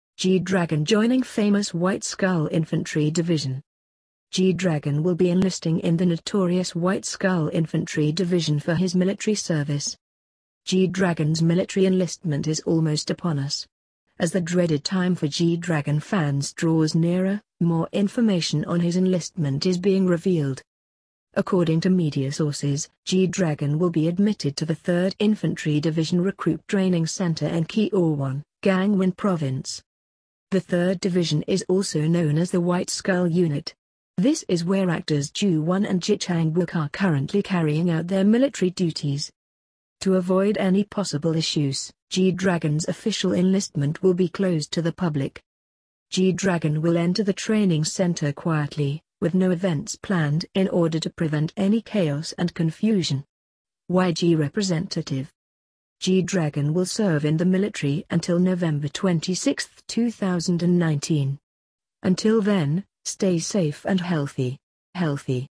G-Dragon Joining Famous “White Skull” Infantry Division
G-Dragon will be enlisting in the notorious ‘White Skull’ infantry division for his military service.

G-Dragon‘s military enlistment is almost upon us. As the dreaded time for G-Dragon fans draws nearer, more information on his enlistment is being revealed.

According to media sources, G-Dragon will be admitted to the 3rd Infantry Division Recruit Training Center in Cheorwon, Gangwon Province.

The 3rd Division is also known as the “White Skull” unit. This is where actors Joo Won and Ji Chang Wook are currently carrying out their military duties.

To avoid any possible issues, G-Dragon’s official enlistment will be closed to the public.

“G-Dragon will enter the training center quietly, with no events planned in order to prevent any chaos and confusion.” – YG Representative

G-Dragon will serve in the military until November 26, 2019. Until then, stay safe and healthy!